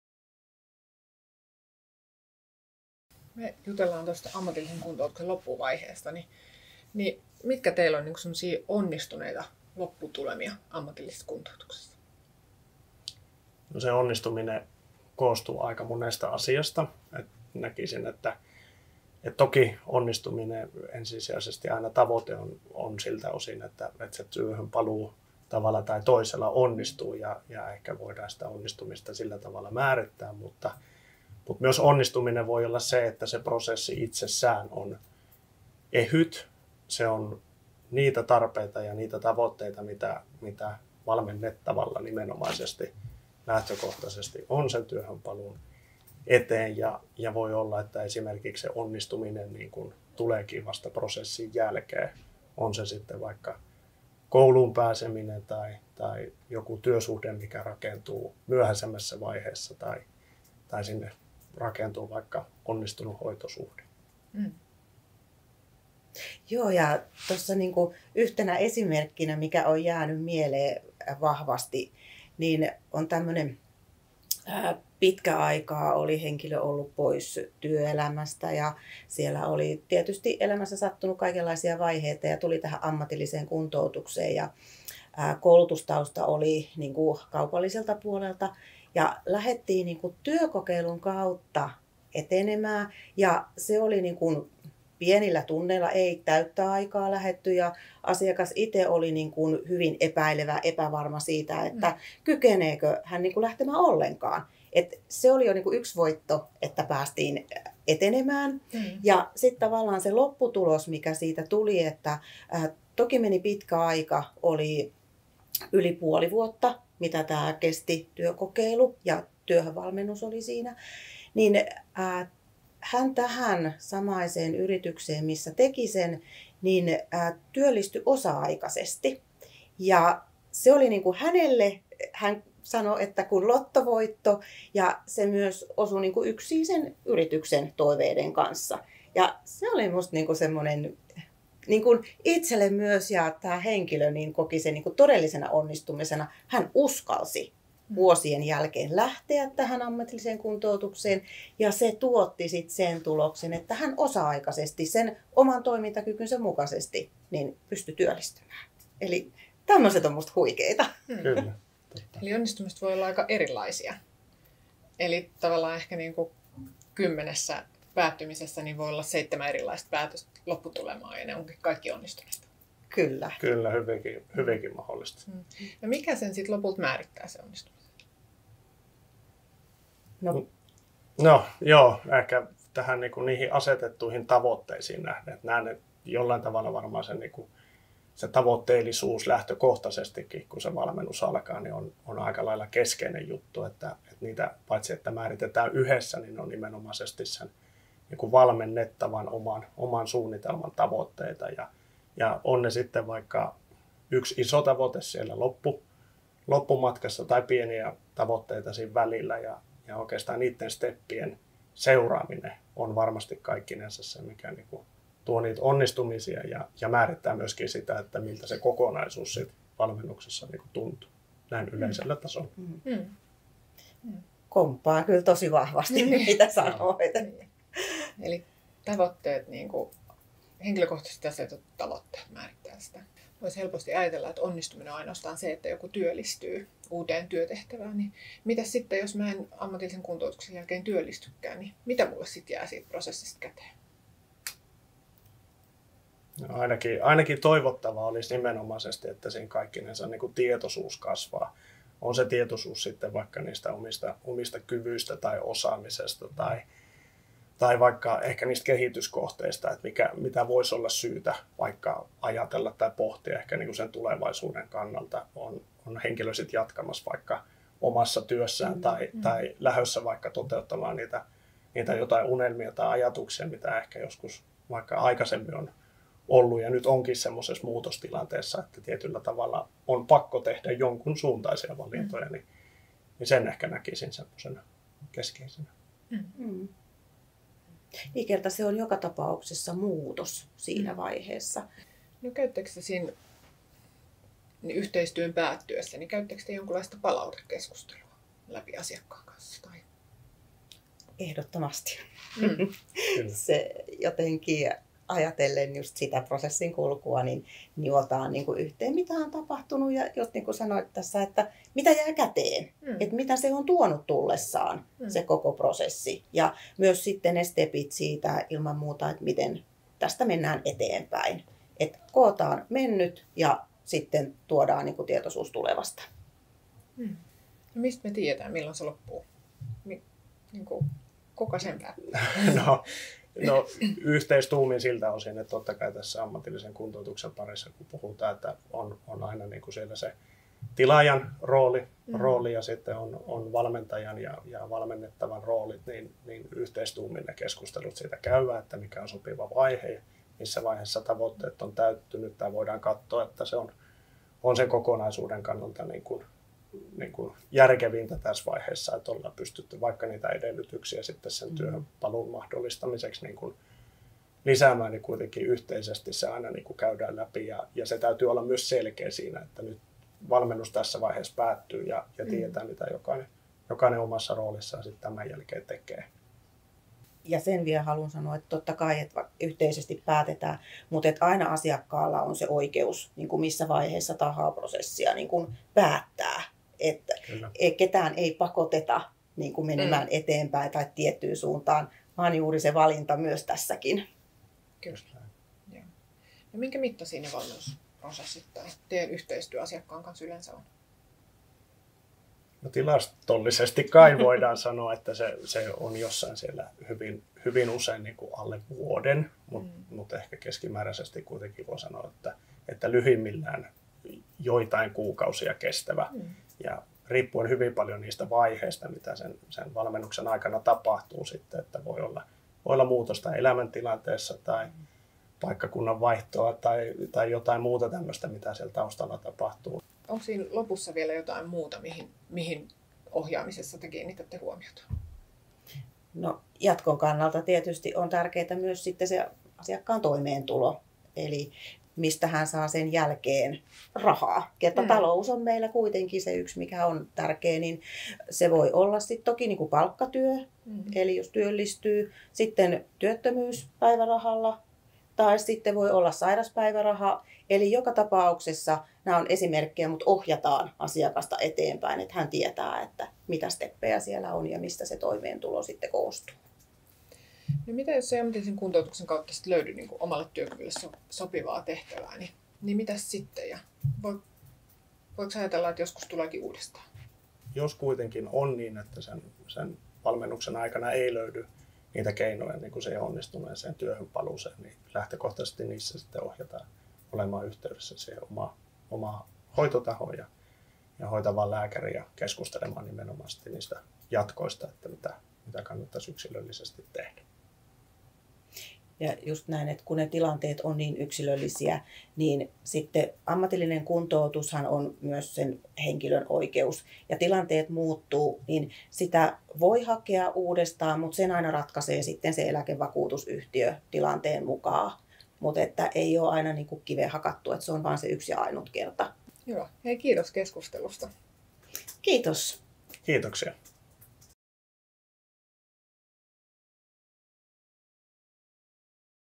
Vaihe 3: Miten ammatillinen kuntoutus päättyy?
Taina Jaakkola, Kim Mononen ja Erja Karhinen keskustelevat ammatillisen kuntoutuksen päättämisestä. Mikä määrittää onnistumisen? Entä, mitä jos sinulle ei löytynytkään työkyvylle sopivaa työtä?